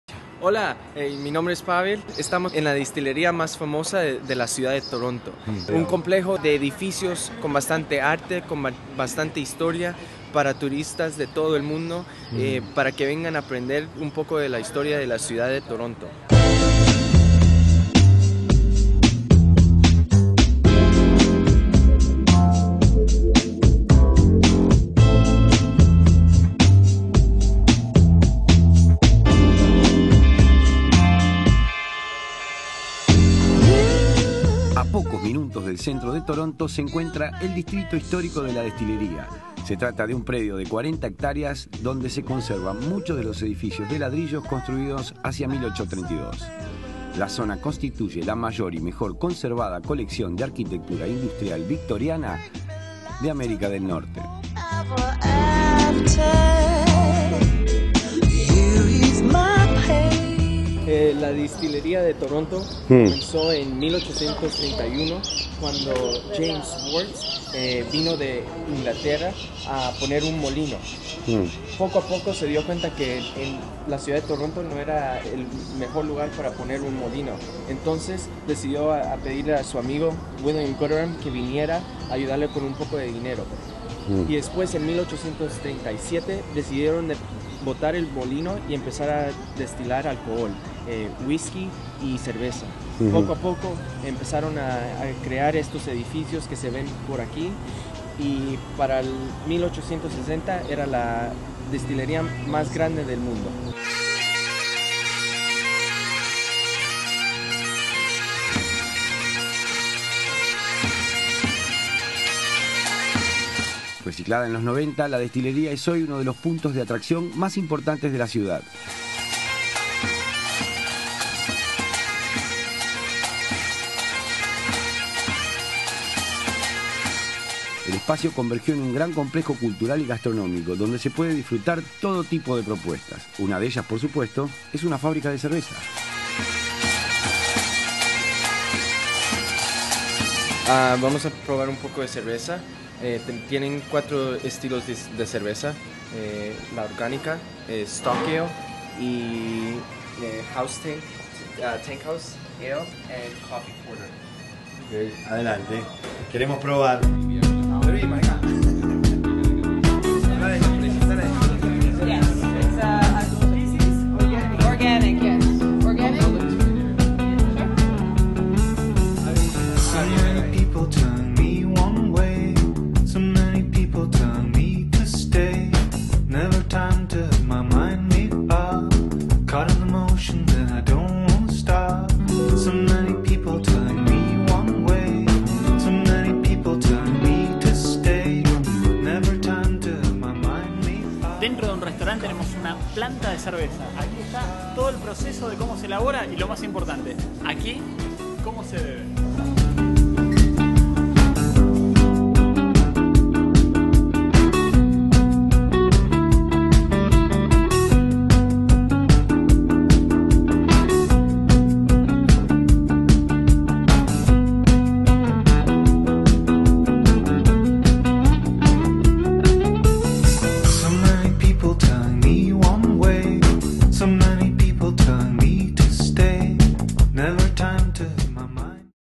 Gooderham and Worts Distillery Toronto - Canadá
En el centro de Toronto por 1800 estaba la  ahora convertido en un lugar cultural y de galerías de arte desde 2001 (Distillery Jazz Festival, Partigras, Roots Music Festival, Antique Festival, Art exhibitions, etc.)
55 Mill Street, Toronto, ON M5A 3C4